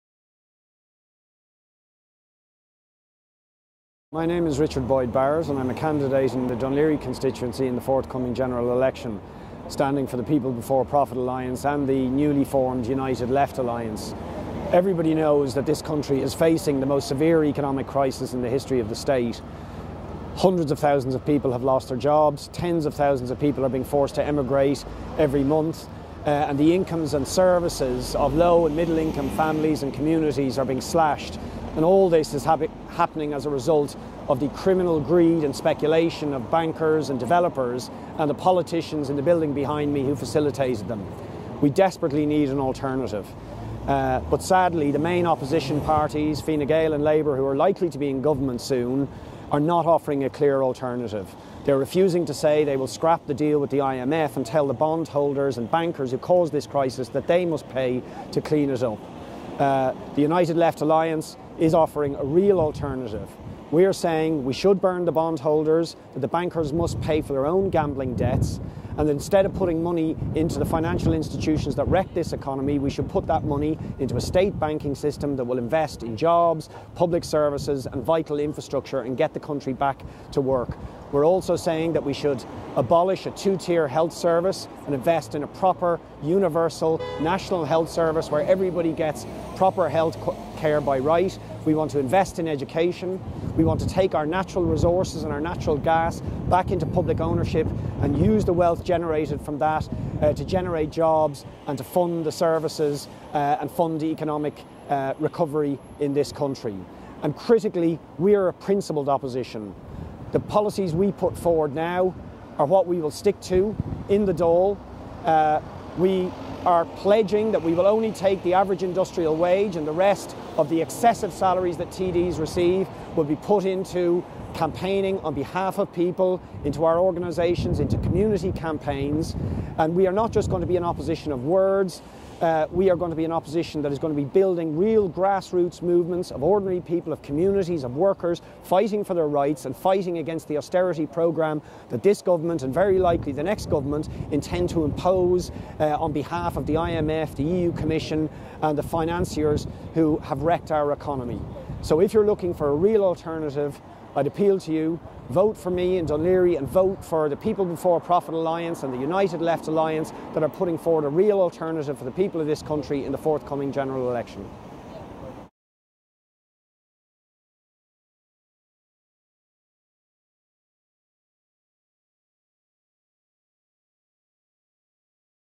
Dún Laoghaire People Before candidate Richard Boyd Barrett at Dail
Dún Laoghaire People Before candidate Richard Boyd Barrett on why he is going up for the Dáil.